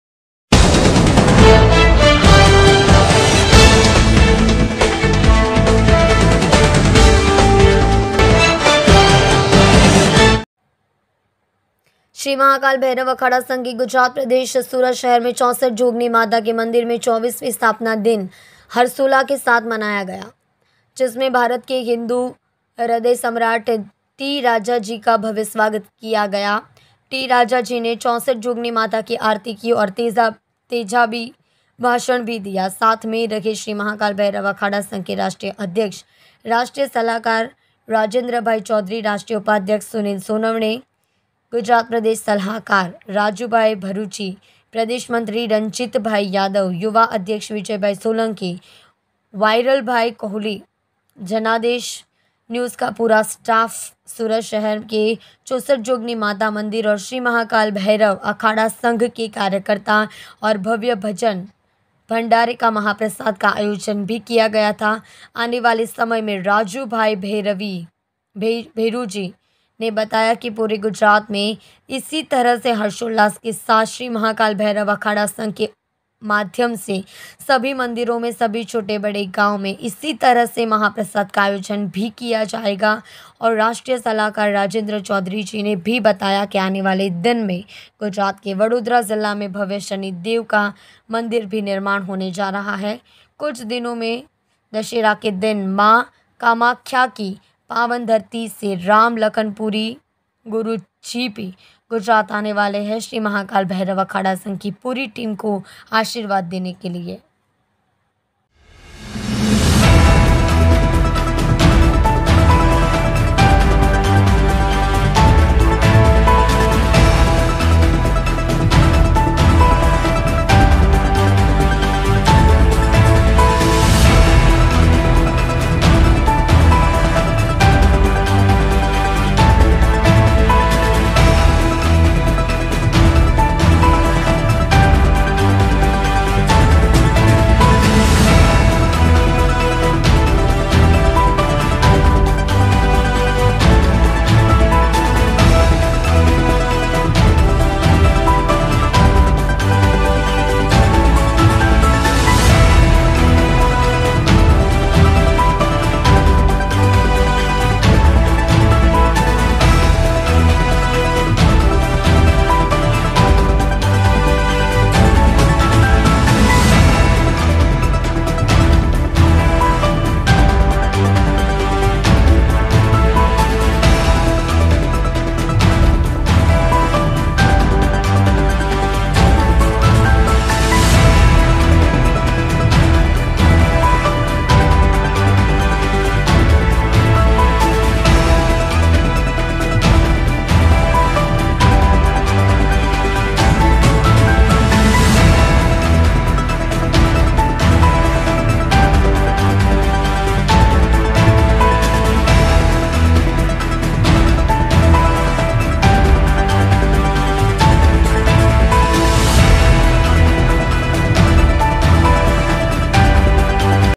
श्री महाकाल भैरव अखाड़ा संघ के गुजरात प्रदेश सूरत शहर में 64 जोगनी माता के मंदिर
श्री महाकाल भैरव अखाड़ा संघ सुशीला चैरिटेबल ट्रस्ट की गुजरात प्रदेश सूरत शहर में 64 जोगनी माता के मंदिर में 24 वे स्थापना दिन हरसोला के साथ मनाया गया जिसमें भारत के हिंदू हृदय सम्राट टी राजा जी का भव्य स्वागत किया टी राजा जी ने 64 जोगनी माता की आरती की और तेजाबी भाषण भी किया साथ में रहे श्री महाकाल भैरव अखाड़ा संघ के राष्ट्रीय अध्यक्ष राष्ट्रीय सलाहकार राजेंद्र भाई चौधरी राष्ट्रीय उपाध्यक्ष सुनील सोनवाने गुजरात प्रदेश सलाहकार राजू भाई भरुचि प्रदेश मंत्री रंजीत भाई यादव युवा अध्यक्ष विजय भाई सोलंकी वायरल भाई गोहिल जनादेश न्यूज़ का पूरा स्टाफ सूरत शहर के 64 जोगनी माता मंदिर और श्री महाकाल भैरव अखाड़ा संघ के कार्यकर्ता और भव्य भजन भंडारे का महाप्रसाद का आयोजन भी किया गया था आने वाले समय में राजू भाई भेरुजी ने बताया कि पूरे गुजरात में इसी तरह से हर्षो उल्लास के साथ श्री महाकाल भैरव अखाड़ा संघ के माध्यम से सभी मंदिरों में सभी छोटे बड़े गांव में इसी तरह से महाप्रसाद का आयोजन भी किया जाएगा और राष्ट्रीय सलाहकार राजेंद्र चौधरी जी ने भी बताया कि आने वाले दिन में गुजरात के वडोदरा जिला में भव्य शनि देव का मंदिर भी निर्माण होने जा रहा है कुछ दिनों में दशहरा के दिन मां कामाख्या की पावन धरती से राम लखन पुरी गुरु जी भी गुजरात आने वाले हैं श्री महाकाल भैरव अखाड़ा संघ की पूरी टीम को आशीर्वाद देने के लिए